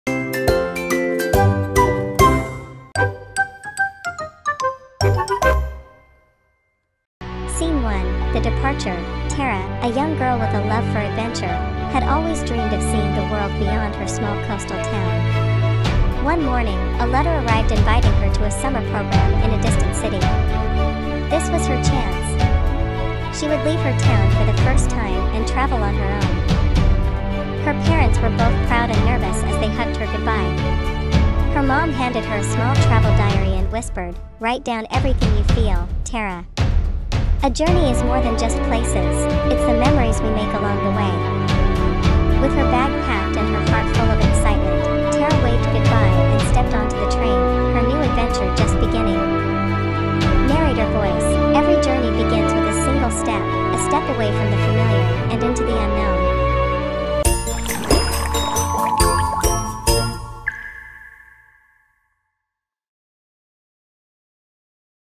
The Journey Begins - The Departure | Kids Venture World
Tara, a young girl with a love for adventure, had always dreamed of seeing the world beyond her small coastal town. One morning, a letter arrived inviting her to a summer program in a distant city.